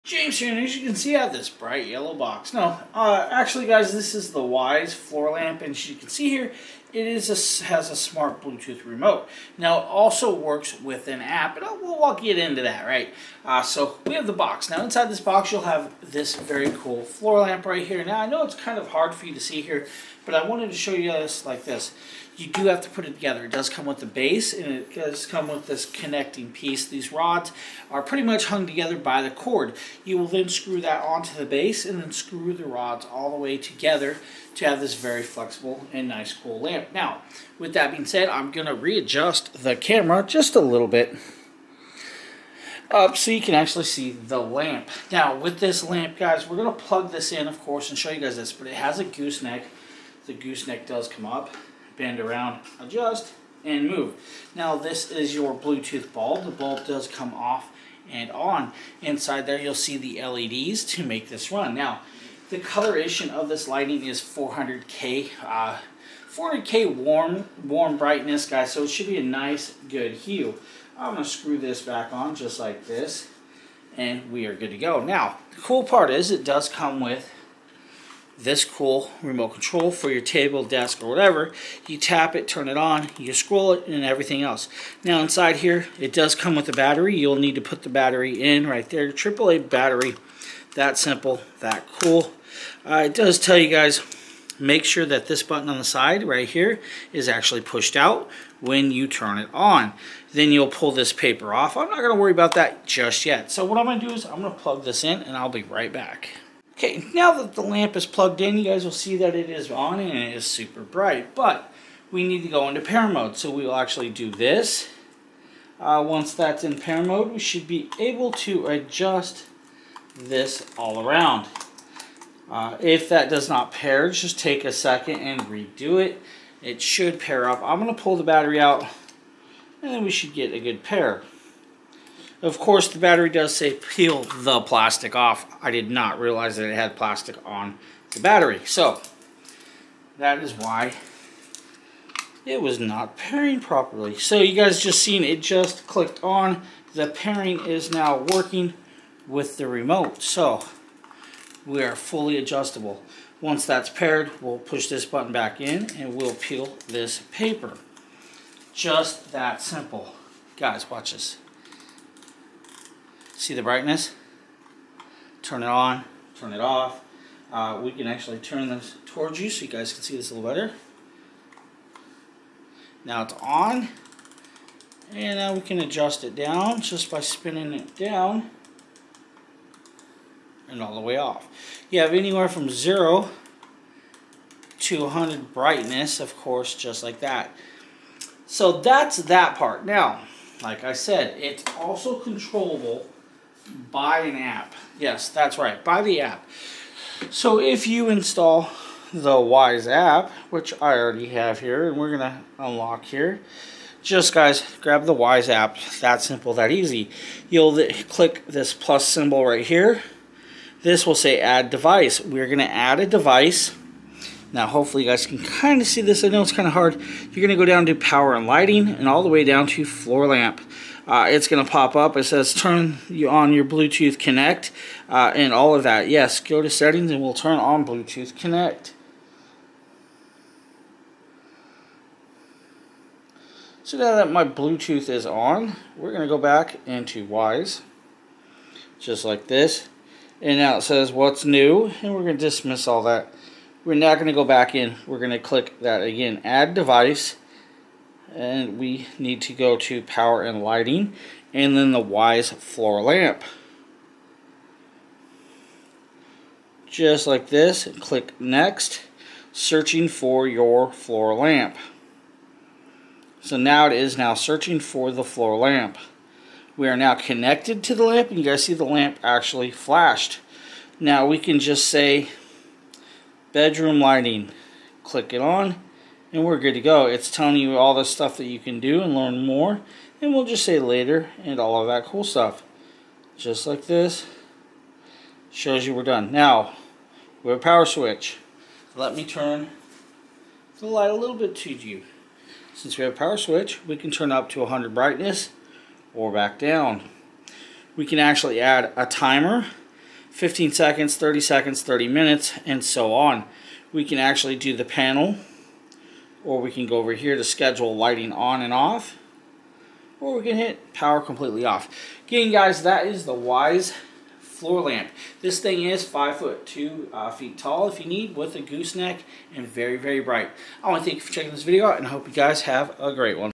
Showcasing WYZE LED Smart Floor Lamp with App Control
Check this out on Amazon

The WYZE smart lamp comes equipped with Samsung LED chips to deliver a powerful brightness level, and cool white light. The lamp is 5'11 with a flexible gooseneck that allows you to freely adjust it how you need, to get the lighting right where you want it. This lamp also has the option to be connected via Bluetooth to the WYZE app, allowing you to control it from there.